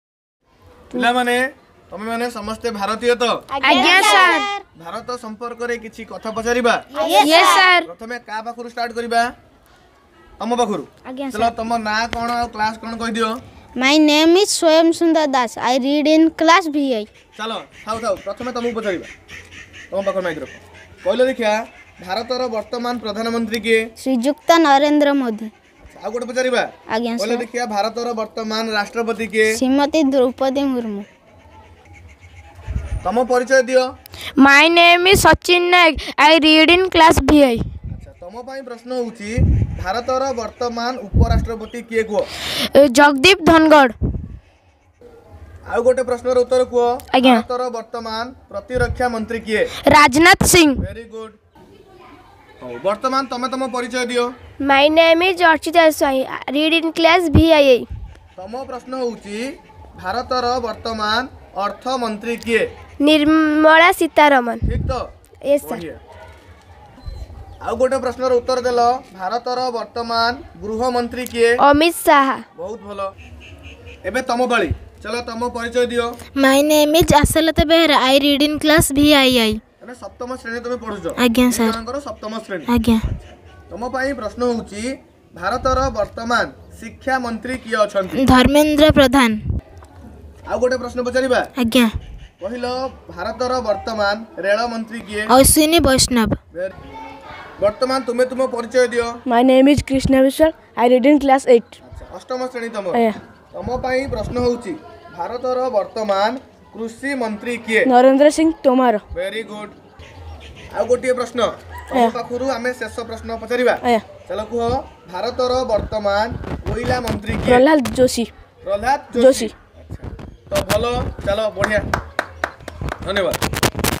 Gk Question answer by Coaching students|Rameswar progress point
Rameswar progress point coaching center
gk question
who is our present president of india
who is our present vice president of india